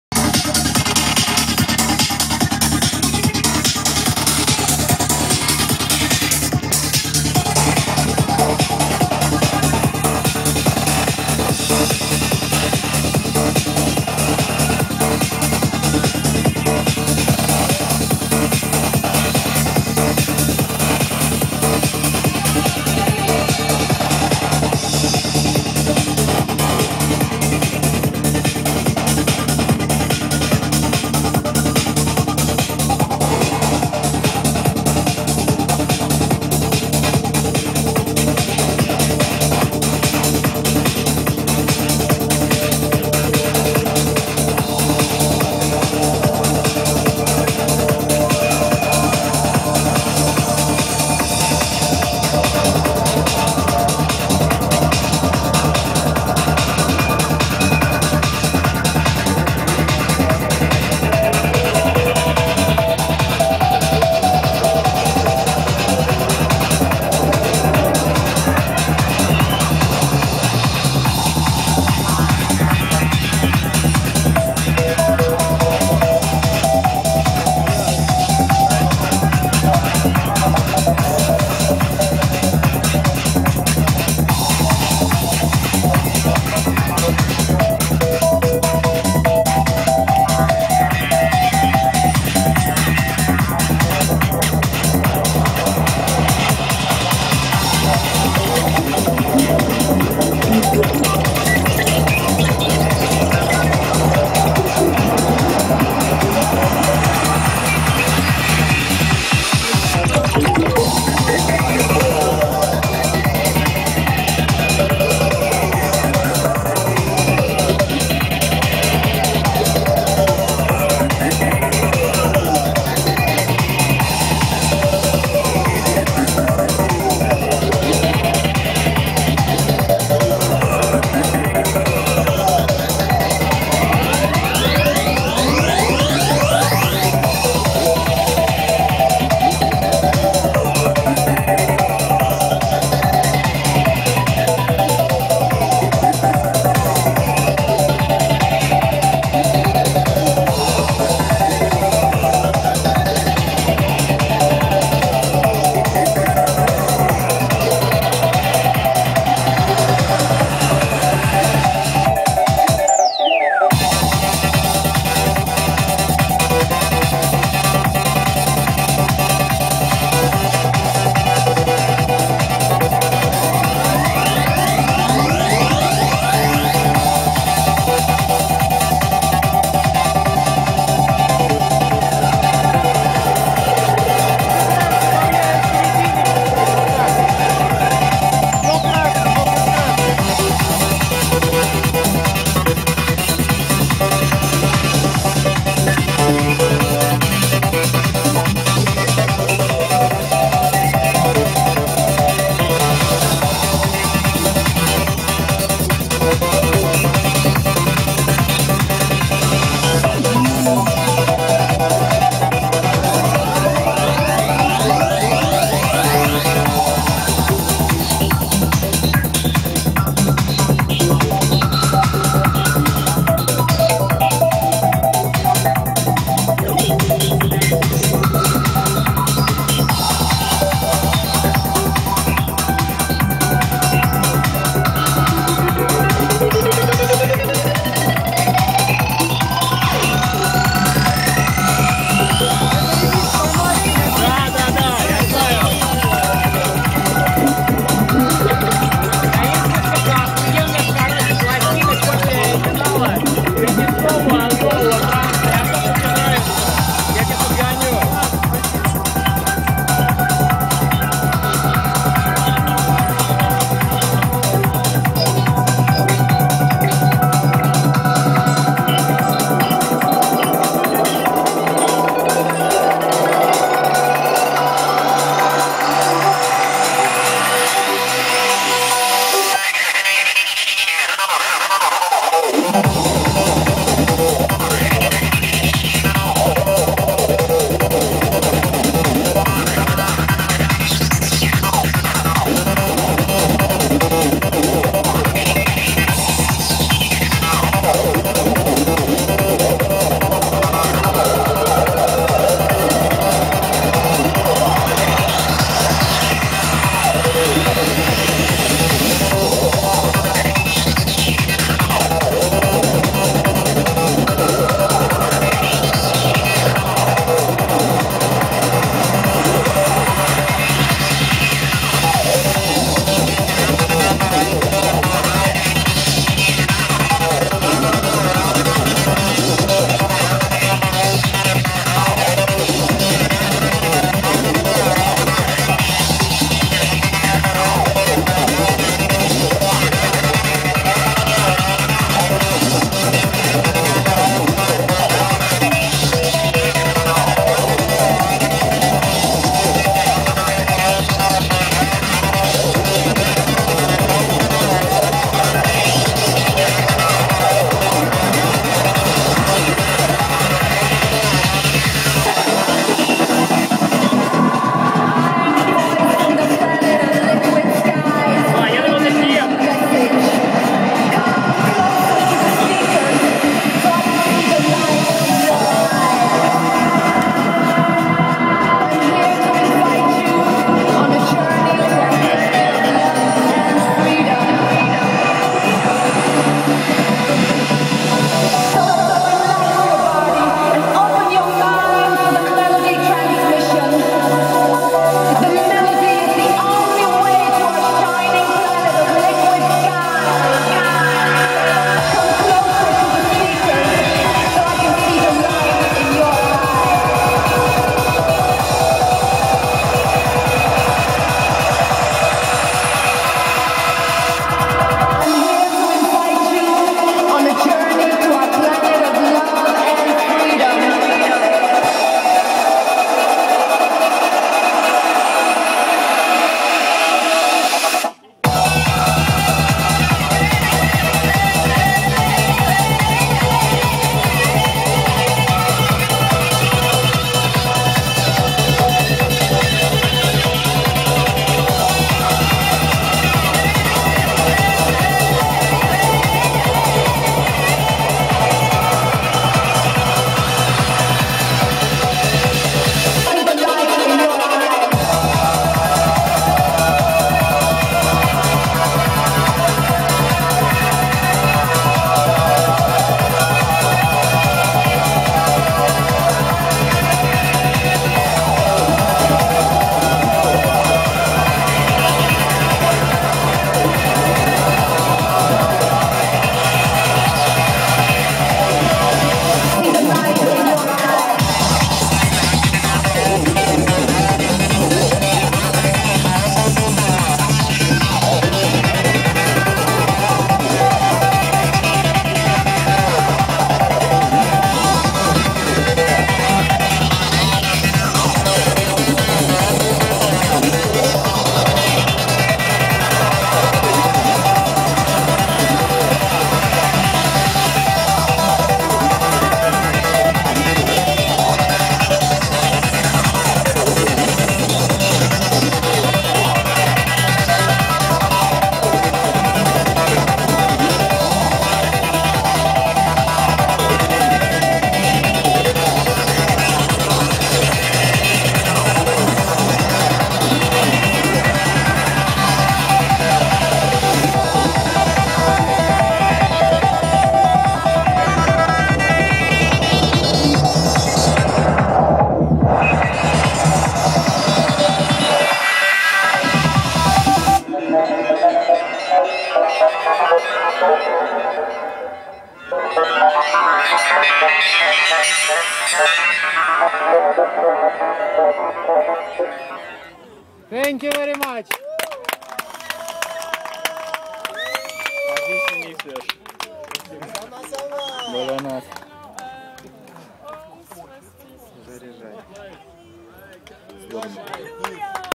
Dj SOLNCE in the Mix @ GC AWAREDANCE 3 Festival, Bali, Indonesia, 04.06.2011. Video part 2
Dj Solnce in the Mix @ GC Awaredance 3 Festival 2011, Kintamani, Mt Batur, Bali, Indonesia, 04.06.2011

Video by Dj Solnce